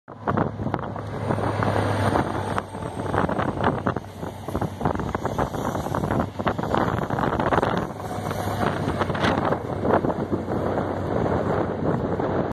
Trains. Short HST (Castle Class) departs Penzance.
Trains. A short HST (Castle Class) departs Penzance with a GWR service to Newton Abbot. April 2019